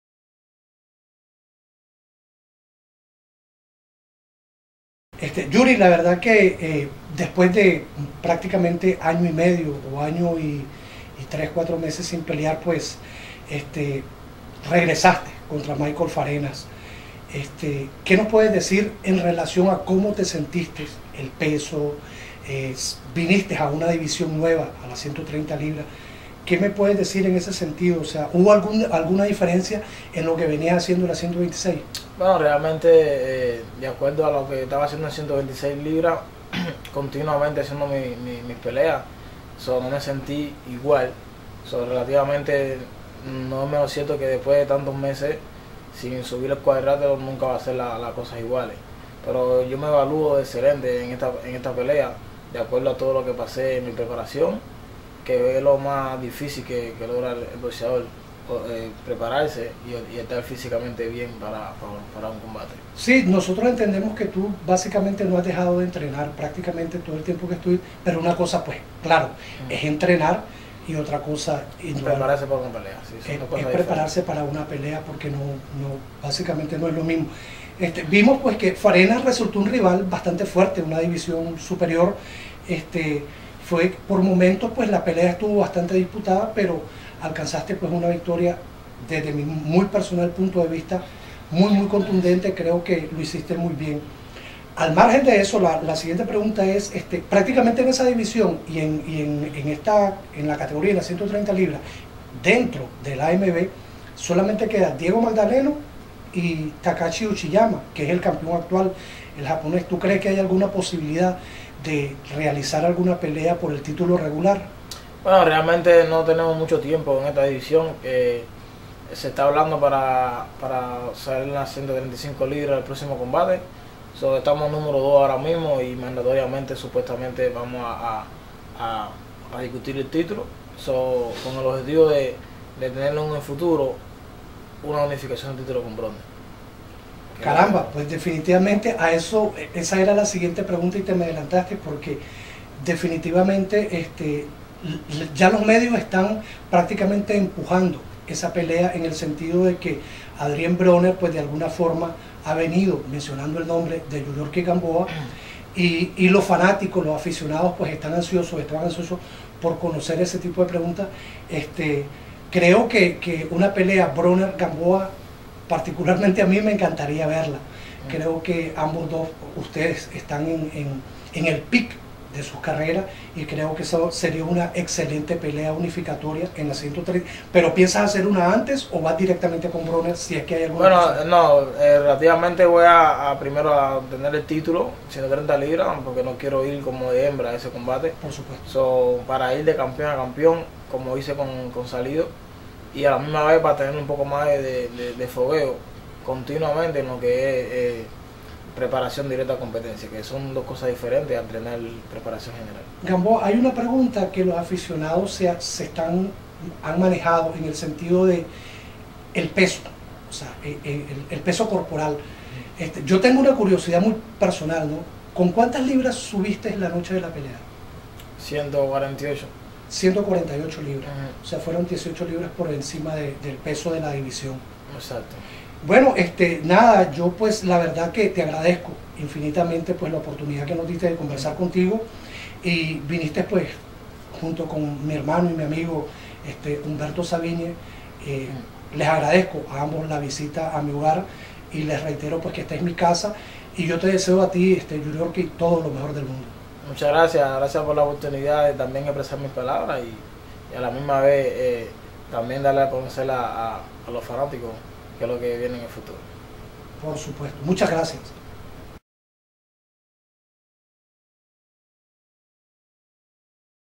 Yuriorkis Gamboa tras los huesos de Adrien Broner
Yuriorkis Gamboa asegura que desea enfrentarse a Adrien Broner de Campeon a Campeon